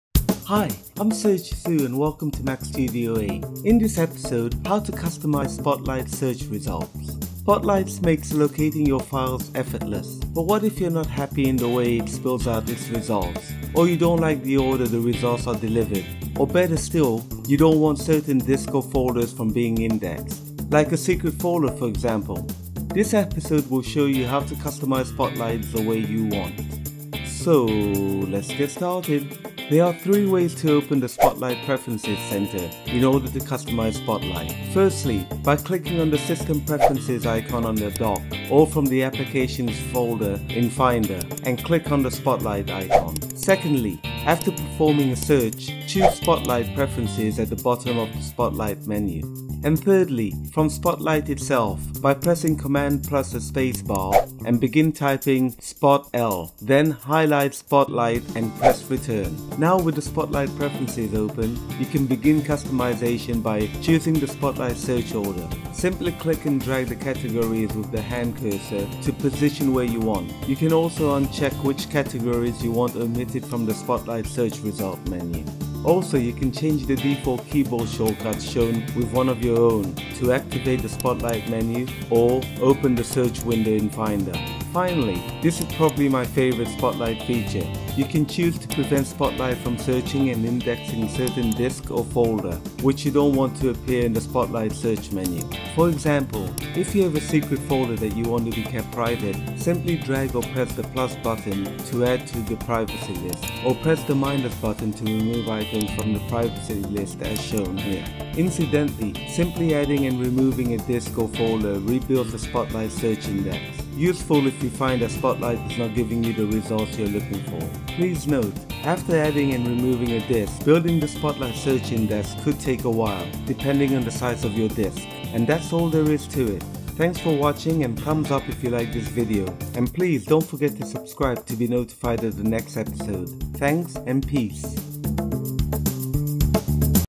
How To Customize Spotlight in Mac OS X Mavericks
How to Customize Spotlight Search Results Using OS X Mavericks and Master Spotlight like a Pro.

Recommended Gear
2TB Thunderbolt and USB 3.0 Portable Hard Drive
4TB USB-C and USB 3.0 Portable Hard Drive
5TB Rugged Thunderbolt / USB-C Mobile Drive

Performed and Tested On MacBook Air (Late 2013) and Using OS X Mavericks 10.9.2

Apple's Intel Based Systems:
MacBook Air
MacBook Pro
Mac Mini
iMac
Mac Pro

RAW SCRIPT:
In this Episode: How To Customize Spotlight Search Results.

Spotlight makes locating your files effortless, but what if you are not happy in the way it spills out it's results or you don't like the order the results are delivered or better still you don't want certain disk or Folders from being indexed...like a secret Folder for example. This Episode will show you how to customize Spotlight the way you want.

There are Three ways to open the Spotlight Preferences center in order to customize Spotlight.

Firstly, by clicking on the System Preferences Icon on the Dock or from the Applications Folder in Finder and click on the Spotlight Icon.

Secondly, after Performing a search, choose Spotlight Preferences... at the bottom of the spotlight menu.

And Thirdly, from Spotlight itself, by pressing cmd + spacebar and begin typing spotl, then highlight Spotlight and press Return.

Now, with the Spotlight Preferences Center open, you can begin the customization by:

Choosing Spotlight search order, simply click and drag the categories with the hand cursor to the position you want.

You can also un-check which categories you want omitted from the Spotlight search result menu.

Also, you can change the Default Keyboard shortcuts shown with one of your own to activate the Spotlight menu or open the search window in Finder.

Finally, this is probably my favourite Spotlight feature, you can choose to Prevent Spotlight from searching and indexing certain Disk or Folder which you don't want to appear in the Spotlight Search menu.

For example, if you have a Secret Folder that you want to be kept private, simply drag or press the plus (+) button to add to the Privacy List or press the minus  (-) button to remove items from the Privacy list as shown here.

Incidentally simply adding and removing a disk or folder rebuilds the Spotlight search Index, useful if you find that Spotlight is not giving you the results you are looking for.

Please note: After adding and removing a Disk, building the Spotlight Search Index could take a while, depending on the size of your disk.